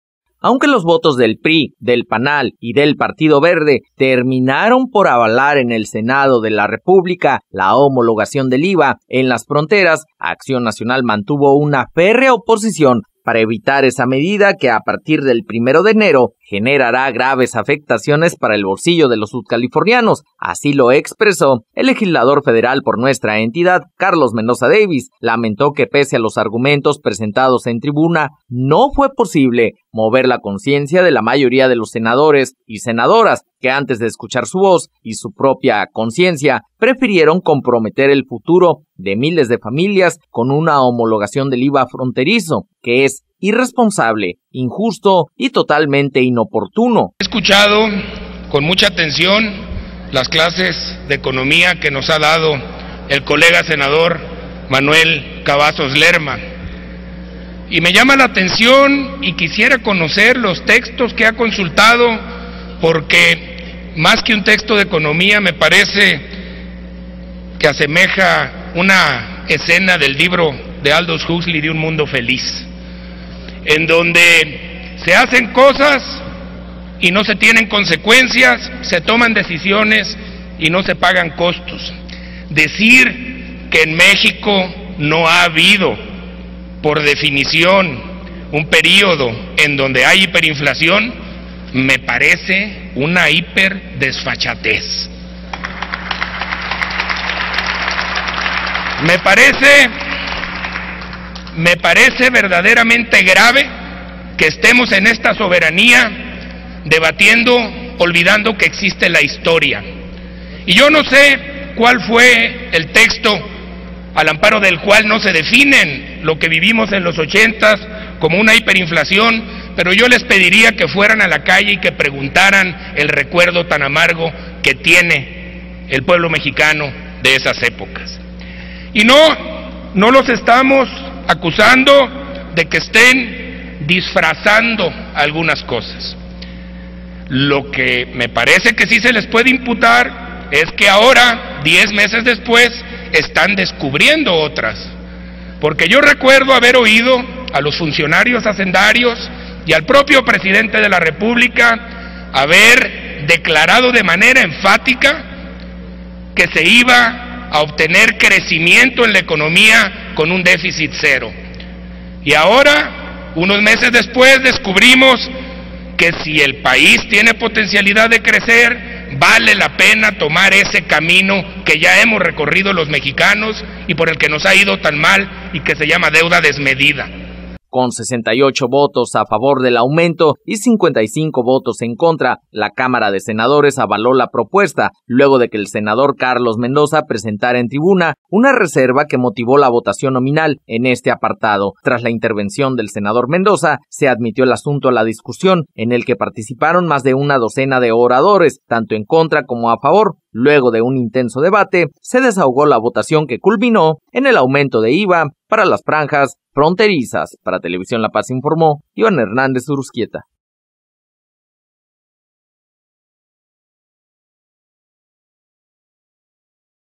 SENADOR CARLOS MENDOZA
EL SENADOR CARLOS MENDOZA DEFIENDE EL NO INCREMENTO AL IVA EN LOS ESTADOS  FRONTERIZOS